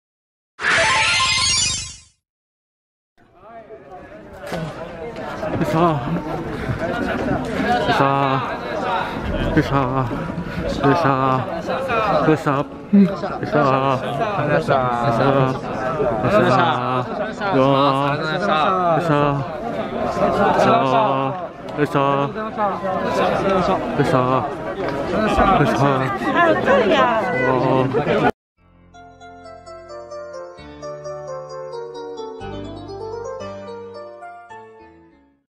【プロ野球⚾】お見送り＠大分B-リングスvs琉球ブルーオーシャンズ2021
「大分B-リングスvs琉球ブルーオーシャンズ」（プロ野球独立リーグ九州アジアリーグ）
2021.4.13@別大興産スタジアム

大分B-リングスの選手が、試合後にお見送り♪
ありがとうございますｍ(__)ｍ
※ピンぼけ失礼いたしますｍ(__)ｍ雰囲気だけでも伝われば幸いですｍ(__)ｍ